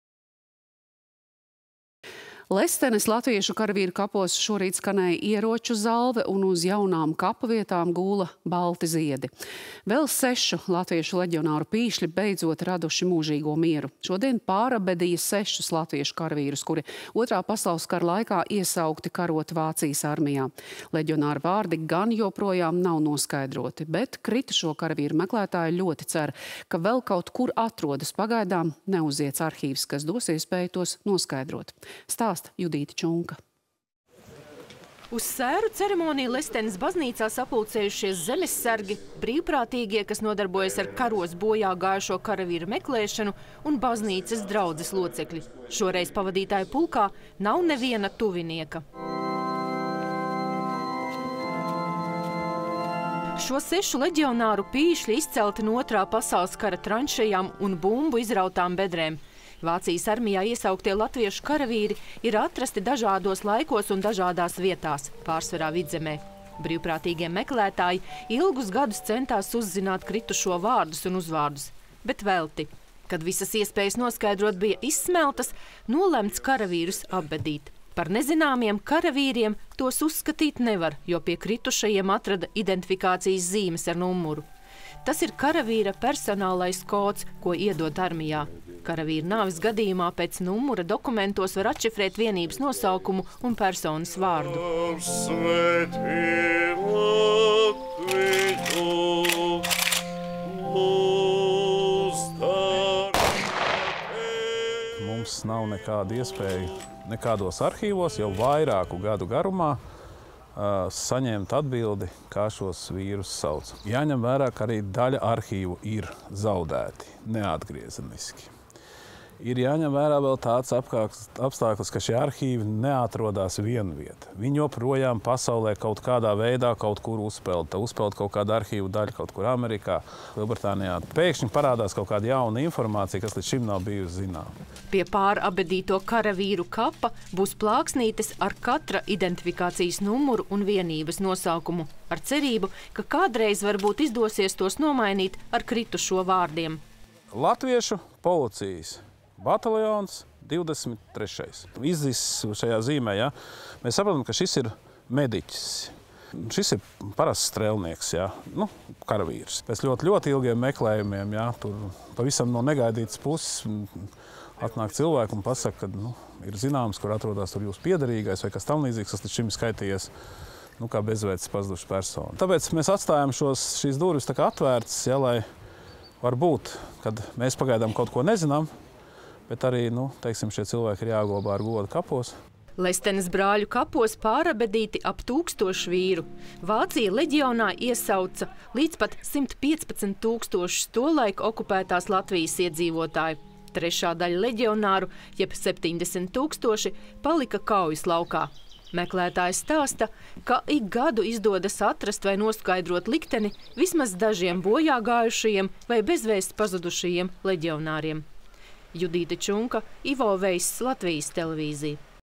Lestenē pārapbedī sešus leģionārus
Lestenes latviešu karavīru kapos šorīt skanēja ieroču zalve un uz jaunām kapa vietām gūla balti ziedi. Vēl sešu latviešu leģionāru pīšļi beidzot raduši mūžīgo mieru. Šodien pārapbedīja sešus latviešu karavīrus, kuri Otrā pasaules kara laikā bija iesaukti karot Vācijas armijā. Leģionāru vārdi gan joprojām nav noskaidroti. Bet kritušo karavīru meklētāji ļoti cer, ka vēl kaut kur atrodas pagaidām vēl neuziets arhīvs, kas dos iespēju tos noskaidrot. Stāsta Judīte Čunka.